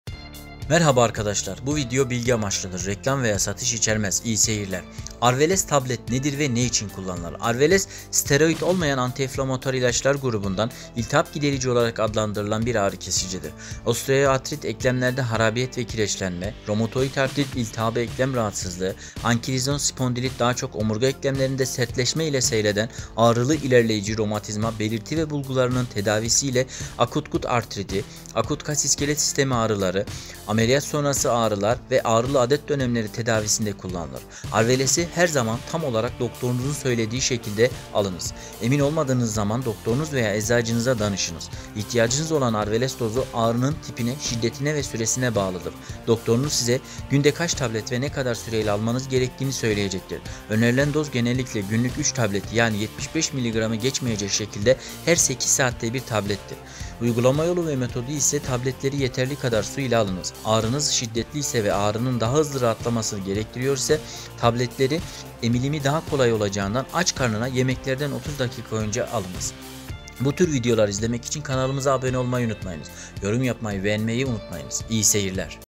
Arveles Tablet Nedir ve Ne İçin Kullanılır...
Arveles, etken maddesi deksketoprofen olan NSAİİ grubu bir ağrı kesicidir. Tablet ve ampul formları vardır. Tablet formu, osteoartrit, romatoid artrit ve ankilozan spondilit gibi kas-iskelet sistemi ağrıları, adet ağrısı, diş ağrısı gibi kısa süreli ağrıların tedavisinde kullanılır.